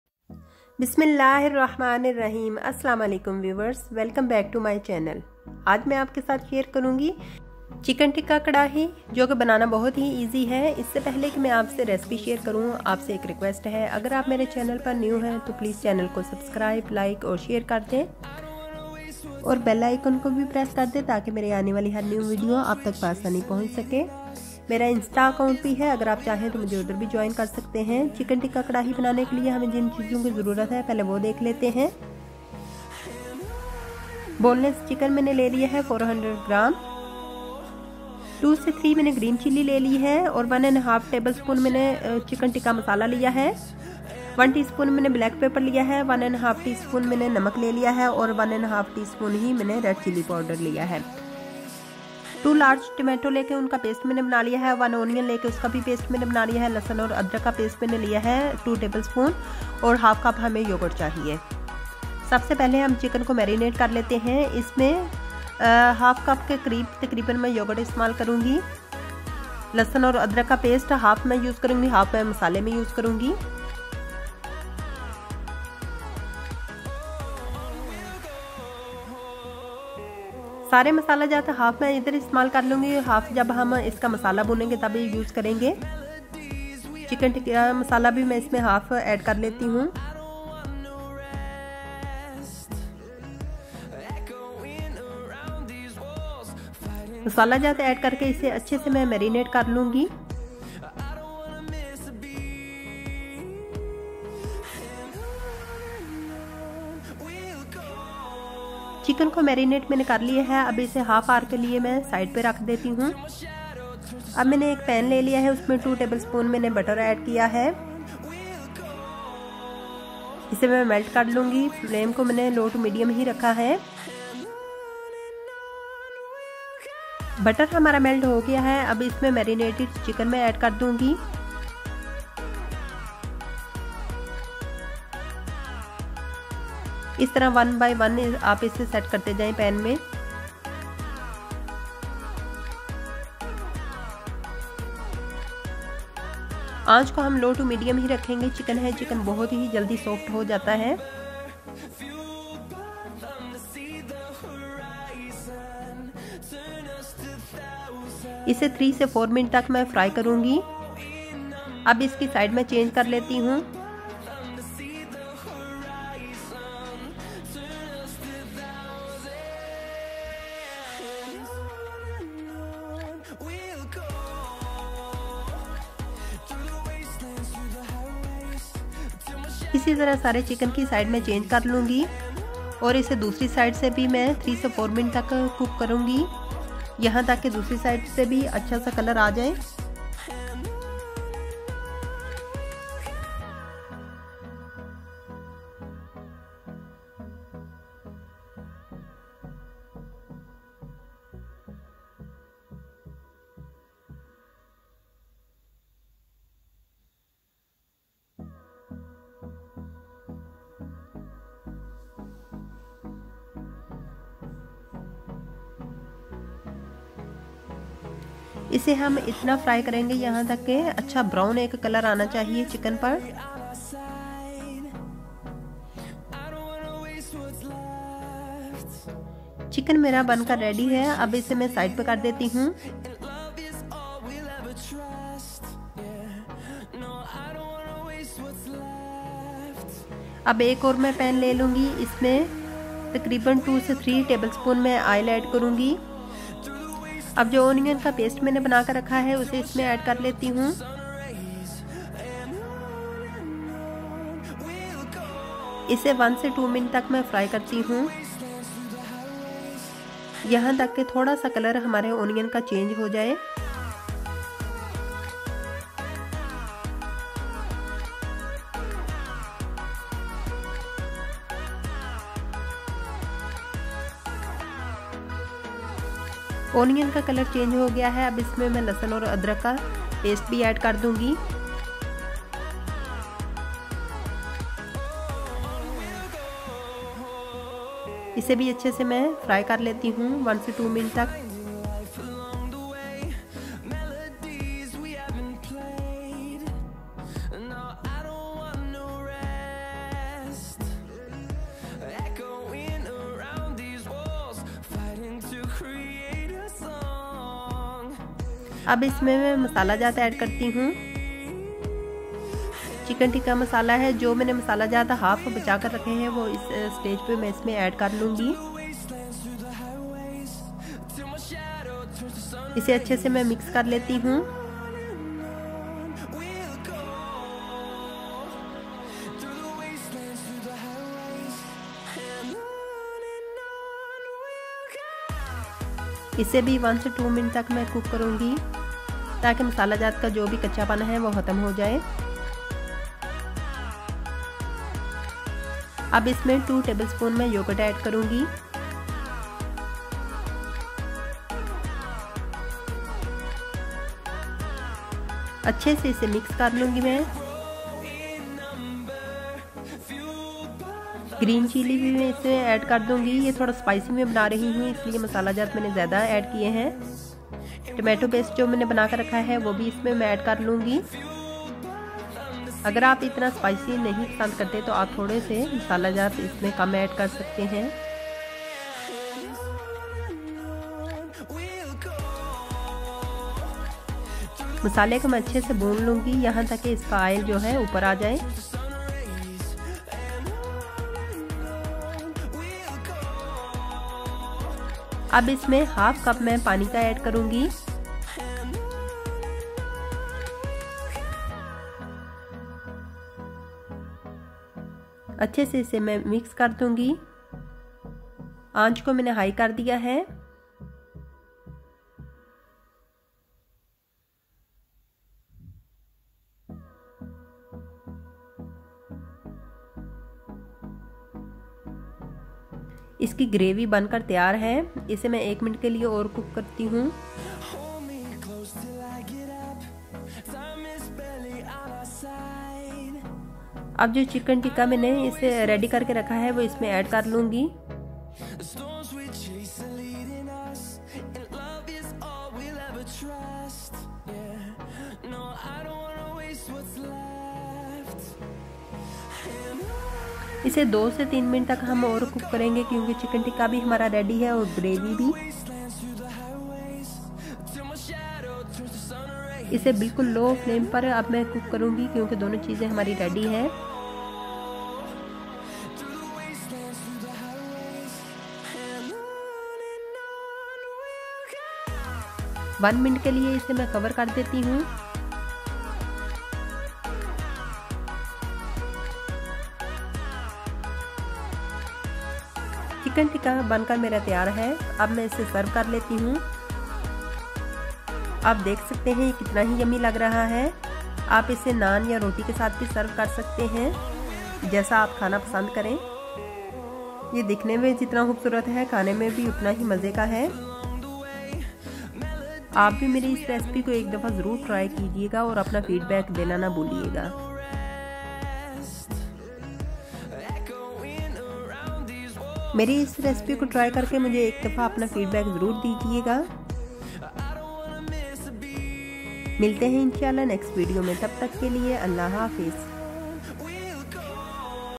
How to Make Easy Chicken Tikka Karahi | Chicken Tikka Karahi recipe | by Fatima kitchen & tips
Description:

How to make Easy Chicken tikka Karahi | Chicken tikka Karahi recipe | Fatima kitchen & tips

ä
Chicken Tikka  karahi recipe

\\ Ingredients //

    haf kg Chicken
   1 onion Paste
   2 Tomatoes paste
   2 tbsp ginger and Garlic paste
  2 green chilli
  2 tbsp tikka Masala
  1 tsp salt
  1tsp red chilli
  1tsp black Pepper
  2 tbsp  butter
  2 tbsp oil
 haf cup Joghurt

Best regards,

Fatima Kitchen & tips